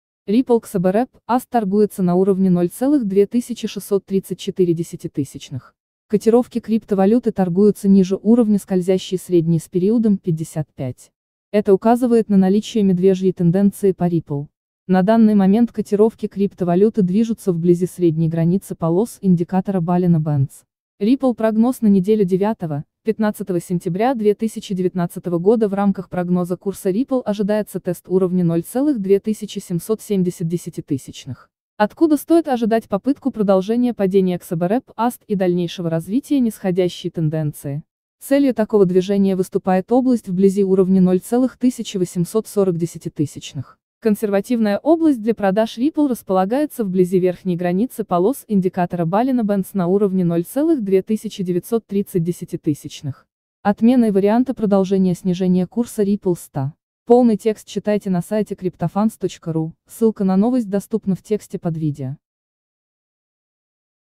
Ripple прогноз на неделю 9 — 15 сентября 2019
Ripple XRP/USD торгуется на уровне 0.2634. Котировки криптовалюты торгуются ниже уровня скользящей средней с периодом 55. Это ука